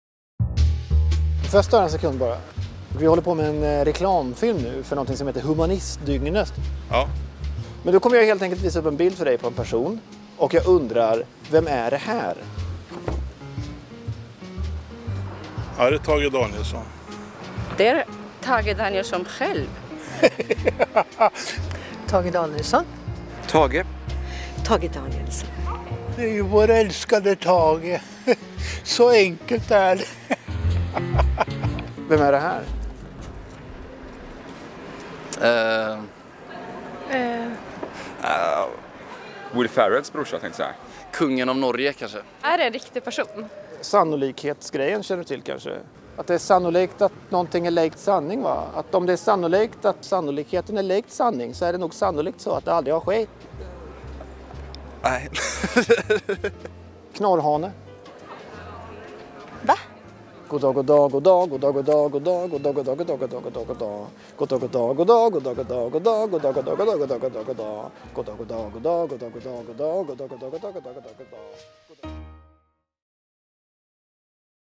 Humanistdygnet 2022
Välkommen till Humanistdygnet 2022!
Då kommer Linköpings universitet, Östergötlands länsmuseum och Linköpings kommun bjuda på en skön blandning av vetenskap och kultur och ge dig tid för nya tankar.

"Vi som gjort filmen heter Elias Dahlgren, Claudio Diez, Marcus Lindgren och Joakim Malmberg.

Vi tog allihop kandidatexamen i Kultur, samhälle och mediegestaltning på LiU i somras. Claudio jobbar nu som frilansande klippare, Joakim på Sjöfartsverket och Elias är frilansande skådespelare och pluggar även vidare på KSMs magisterutbildning tillsammans med Marcus.

Under studierna så inriktade vi oss främst på videoproduktion och då specifikt inom  humorgenren. Bland annat producerade vi humorserien "BRÖL" som nu ligger ute på Youtube.

Under vårt sista år skrev vi en uppsats som kretsade kring Hasse & Tages humorprogram "Mosebacke Monarki" från 1968 och vi valde att ta inspiration från våra analyser av sketcherna i programmet när vi skrev manus till reklamfilmerna för att ta fasta på Humanistdygnets nära relation till Tage Danielsson."